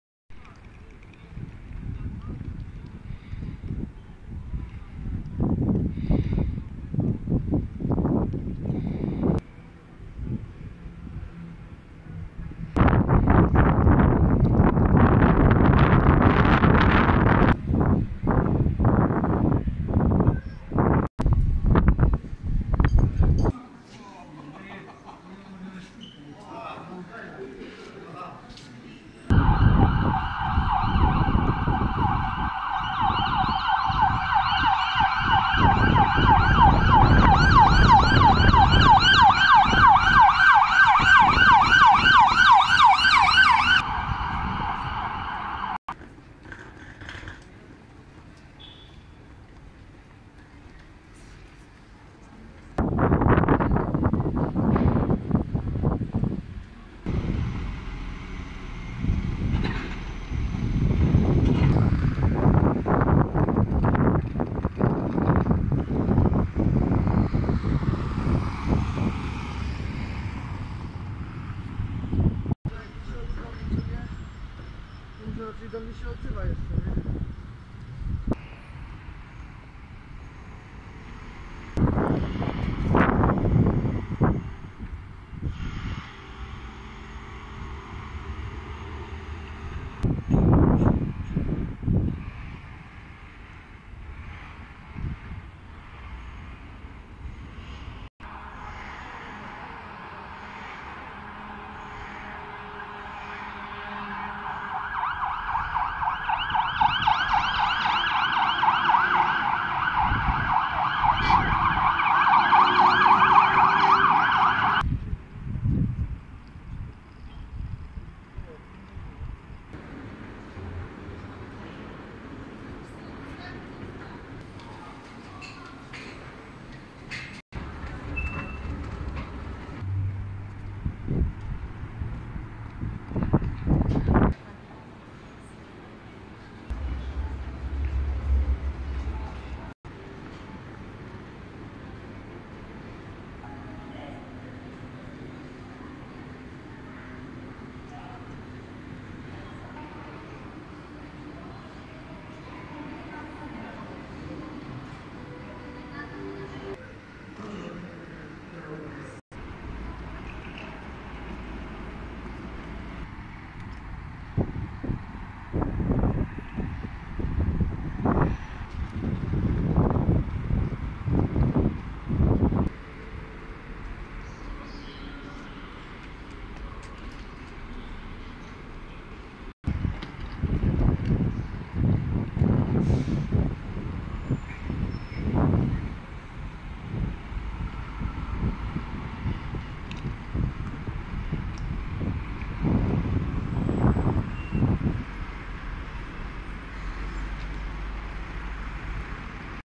Koronawirus Kraków sytuacja na dzień 13.03.2020
Tymczasem w Katowicach podejrzenie zachorowania
miałby nim być pracownik Uniwersyteckiego Centrum Klinicznego im. prof. Kornela Gibińskiego Śląskiego Uniwersytetu Medycznego w Katowicach. Informację tę potwierdziła pełniąca obowiązki rzecznika szpitala Joanna Chłądzyńska. Aktualnie najliczniesze w Kraju zachorowania Racibórz 5 osób, Ostróda 4 (jedyne miasto w województwie warmińsko-mazurskim stan na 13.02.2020 rano) Warszawa 6.